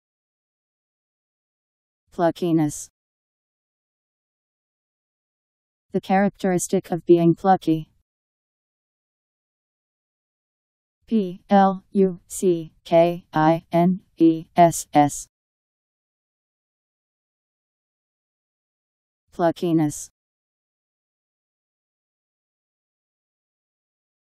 Pluckiness Meaning
Video shows what pluckiness means. The characteristic of being plucky..  Pluckiness Meaning. How to pronounce, definition audio dictionary. How to say pluckiness. Powered by MaryTTS, Wiktionary